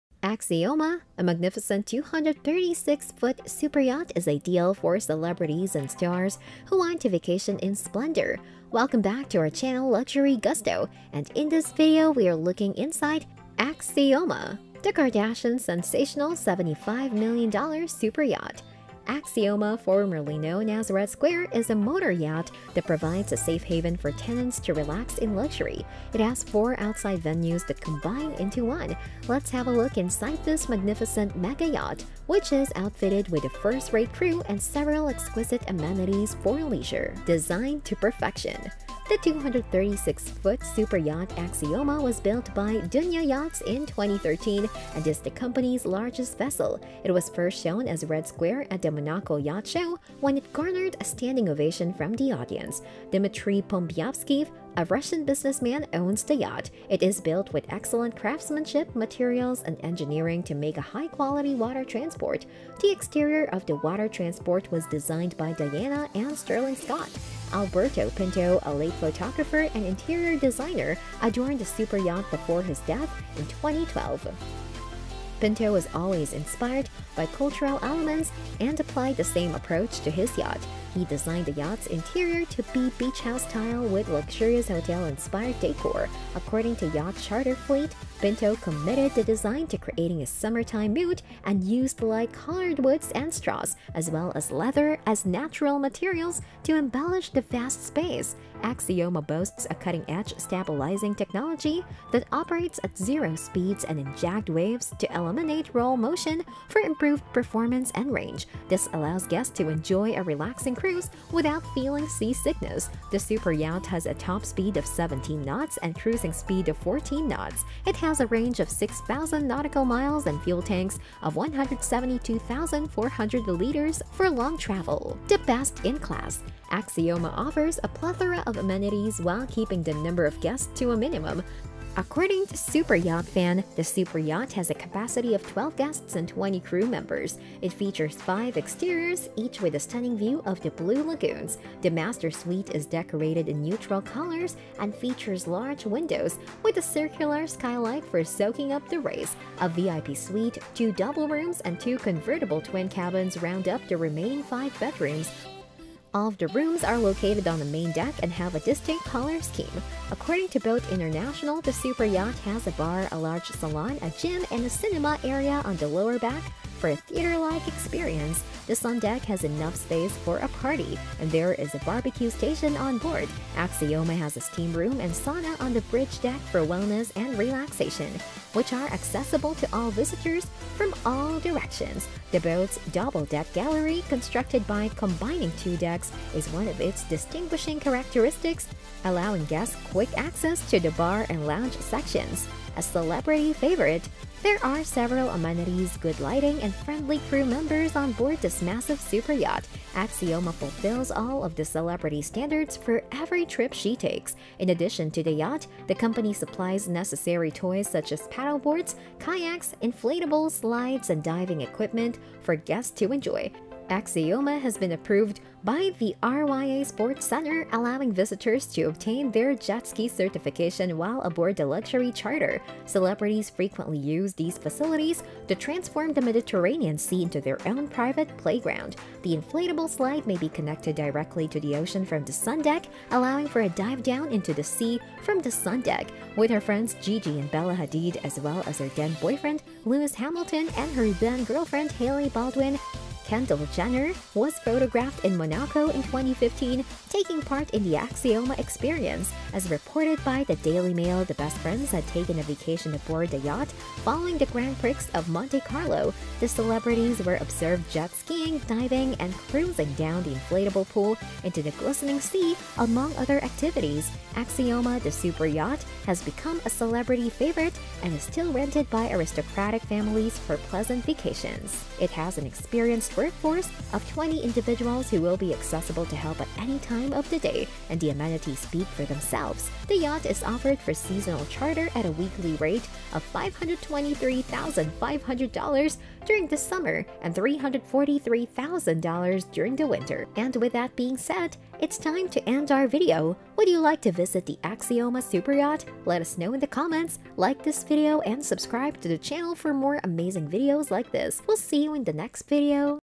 Inside Axioma The Kardashians' Sensational $75 Million Superyacht
In today's Luxury Gusto video we are going to be looking Inside Axioma The Kardashians' Sensational $75 Million Superyacht!